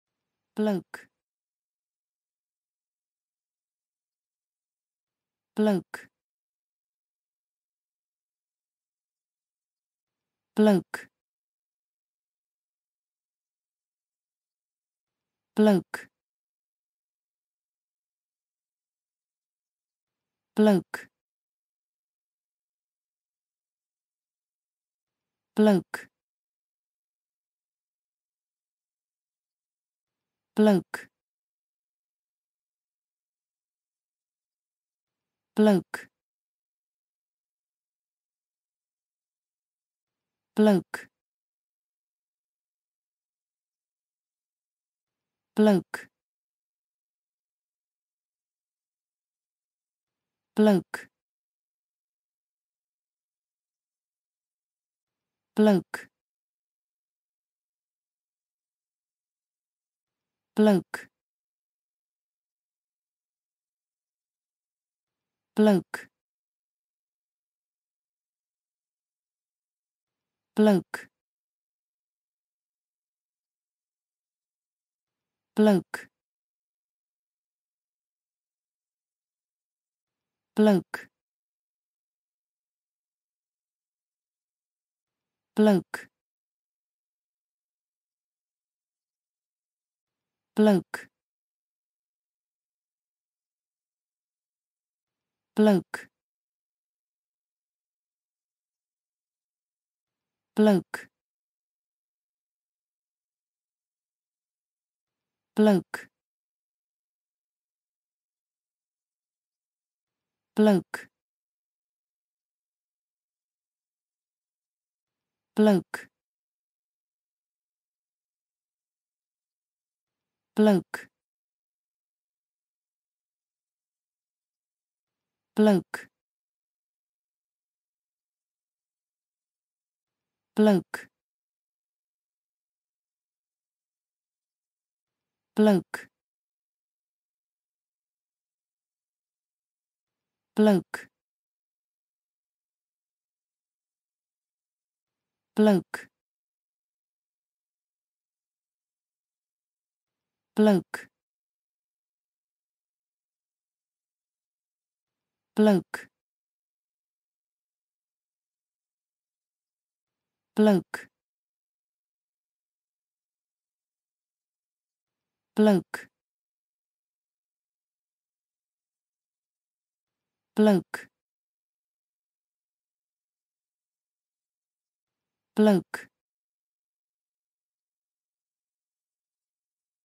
bloke. How to pronounce the english word bloke .Start with B. Learn from me.
Follow me!Learn from me how to pronounce the english word bloke.This video shows you how to pronounce the English word bloke .